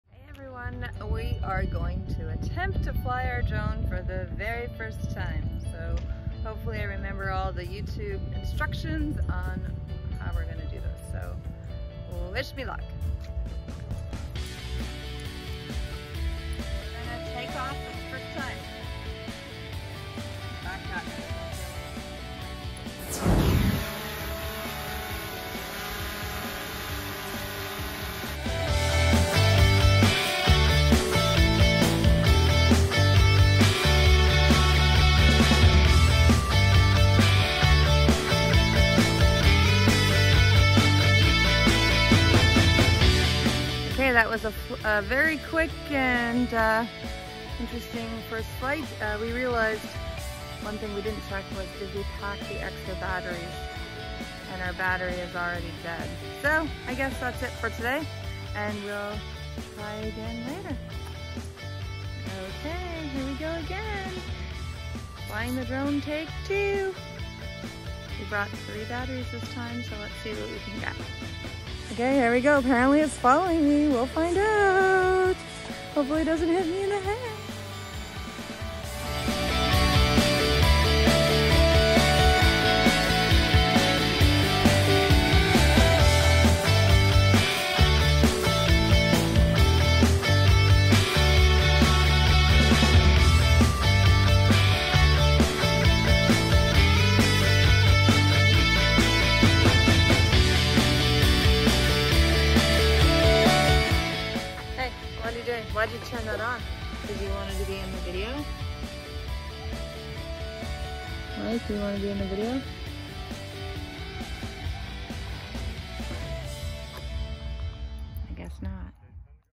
First time flying the drone
We passed! Mike and I are officially pilots. So we figured we better figure Mavic out. I think his (the drone ) should be named Maverick, because that is what I keep calling him.

Follow us for more helpful tips on flying a drone….🤣🤣🤣🤣🤣🤣